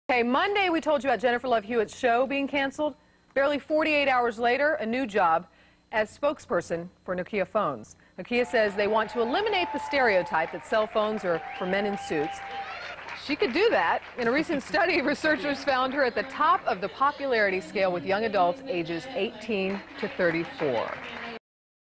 Jennifer Love Hewitt (FOX News,Lisa Stanley Item) June 2000
This short clip came by just after "Time of Your Life" was cancelled.

JENNIFER LOVE HEWITT then,for a short time,became a spokesperson for "Nokia" cell phones.